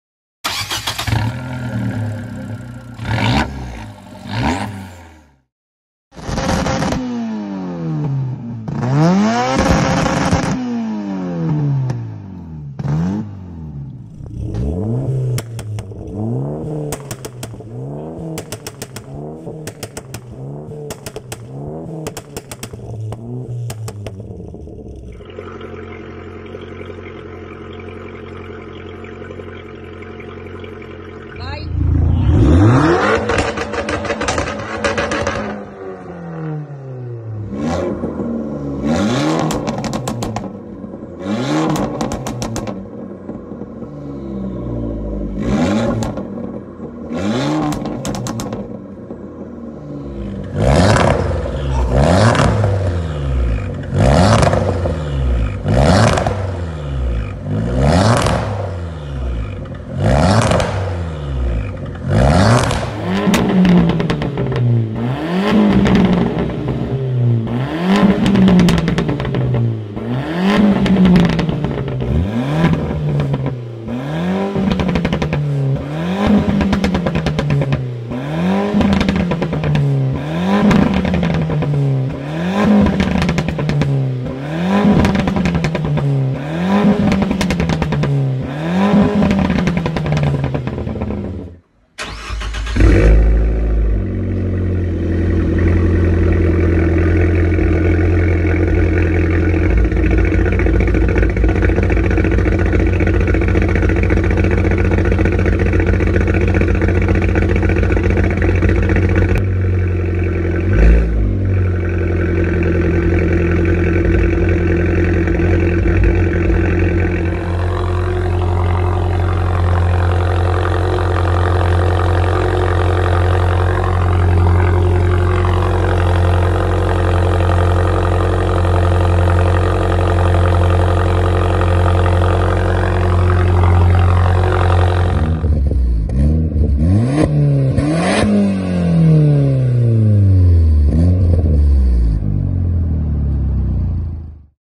MAKE SOME NOISE ep. 13 / Abarth sound compilation
Episodio 13 del progetto "make some noise" in cui raggruppiamo il sound delle nostre Abarth nelle loro diverse configurazioni (skat, stock, bang, Akrapovic, monza, ecc).

Se volete partecipare ed essere inclusi nella compilation mandateci il vostro video!

Un ringraziamento in questo video a OldSchoolGarage (Cesare), jokeralfe, _abarth57_, valeria_carbonaro, abarth_yellow_595, CoccosGarage, Lucaabrth93, Daniele, Fabio, w-style